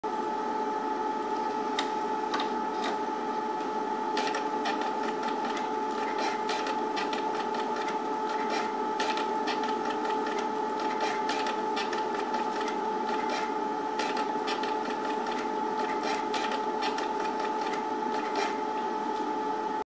ColourWise Multi Colour Trophy Inserts - Epson C3500
A little demonstration of the kind of fill effects you can produce using the Epson C3500